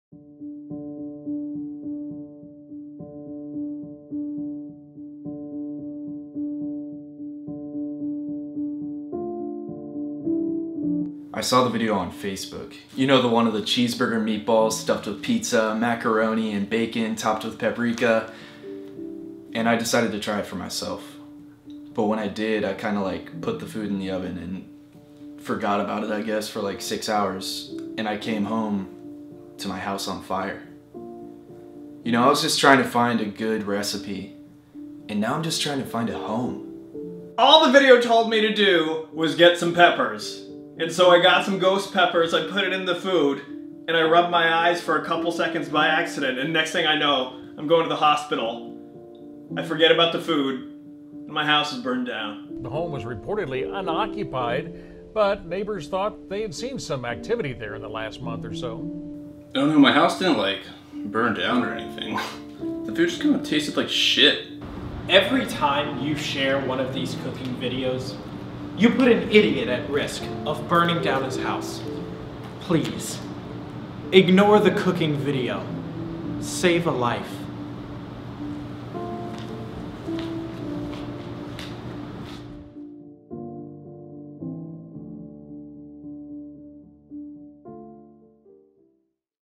StopBuzzFeed2016 - Cooking Video PSA
"Tasty" cooking videos on Facebook have gone too far and they must be stopped.

Please support the movement by sharing this video and tweeting